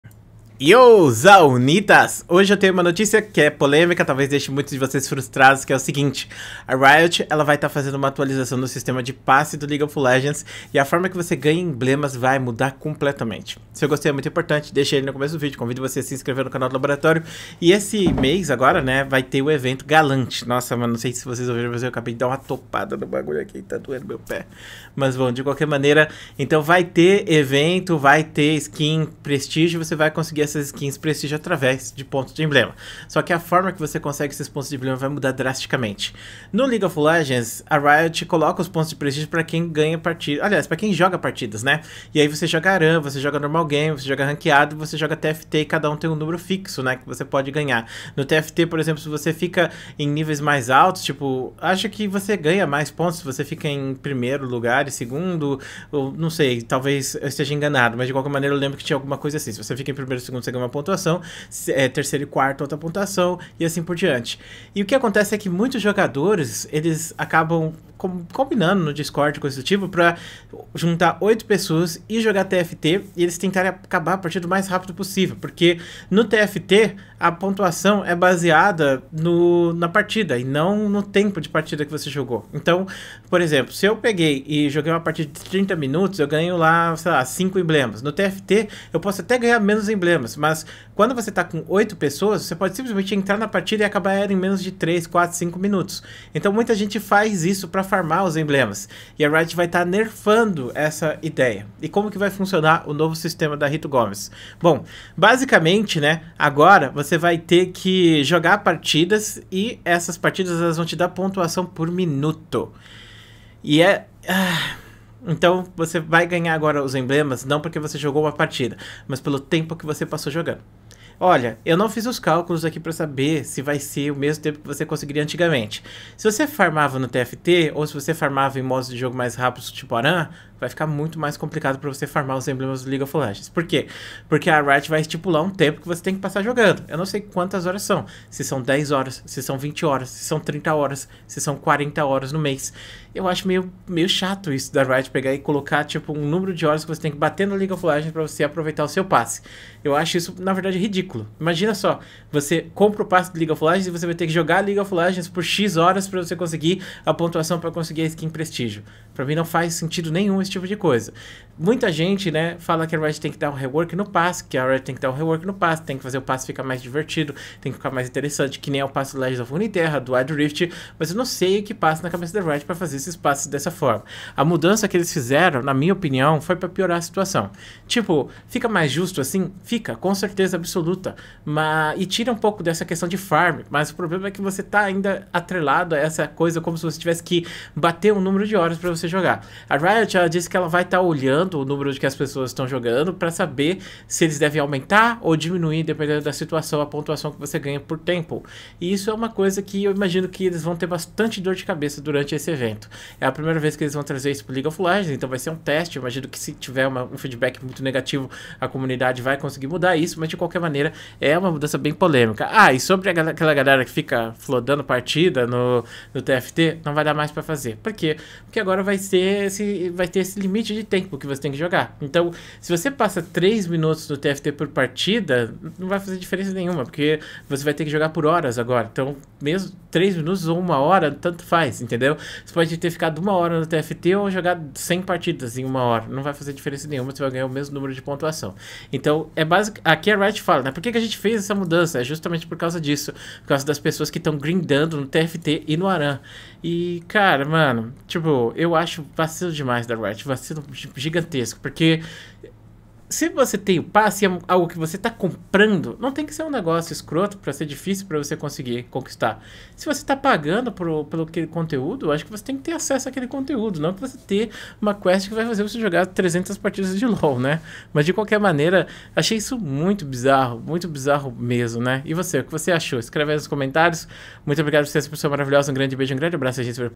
SÉRIO? O PASSE DO LEAGUE OF LEGENDS AGORA VAI SER ASSIM?
que isso clã, pra que isso? agora o passe vai te recompensar por horas jogadas?